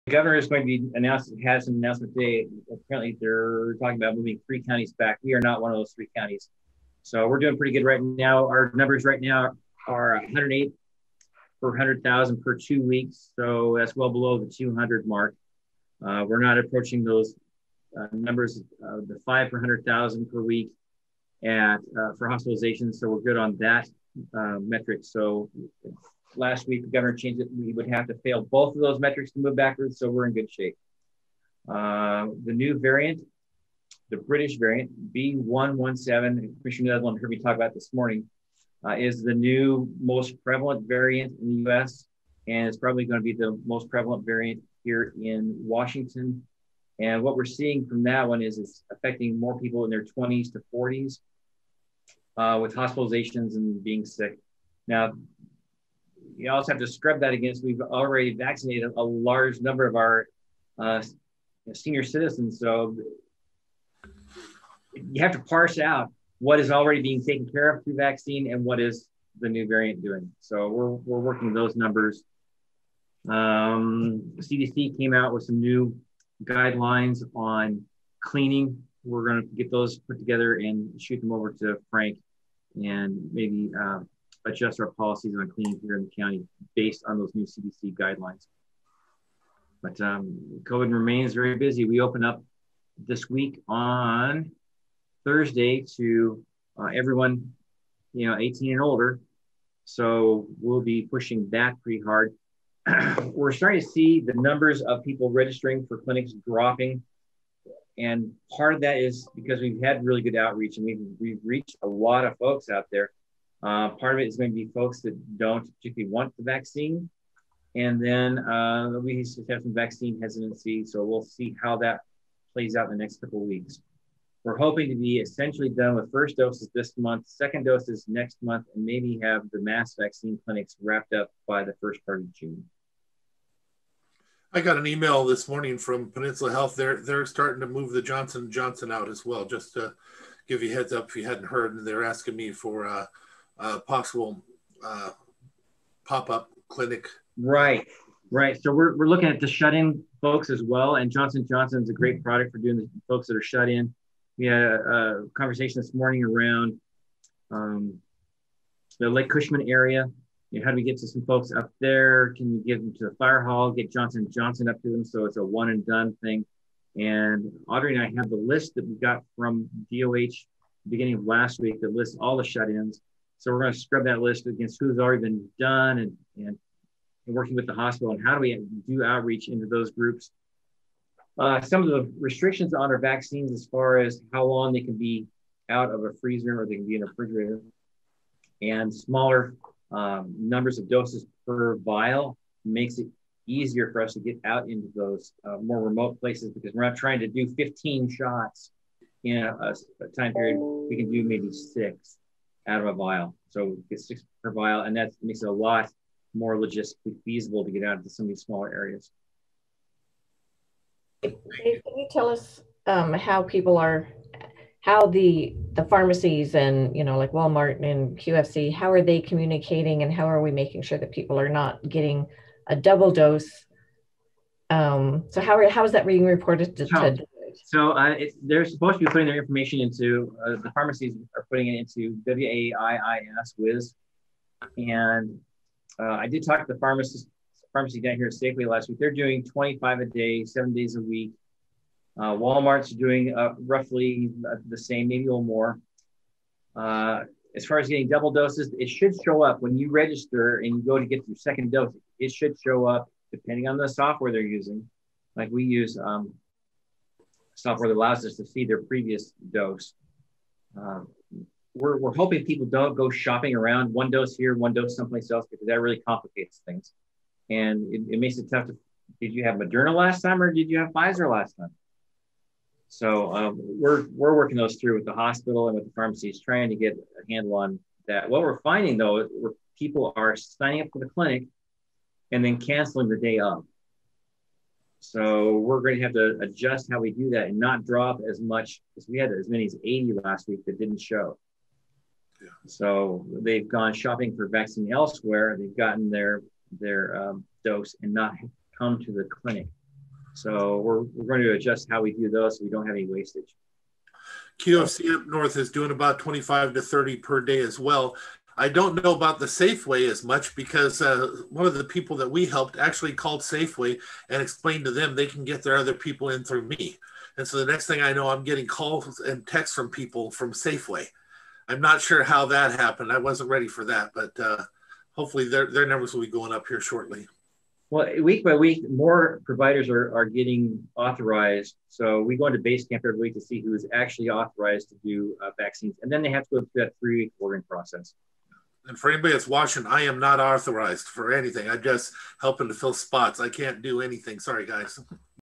COVID-19 Update April 12, 2021
Update on COVID-19 in Mason County from Dave Windom, Director of Mason County Community Services which includes Mason County Public Health, during the April 12, 2021 Mason County Commission briefings.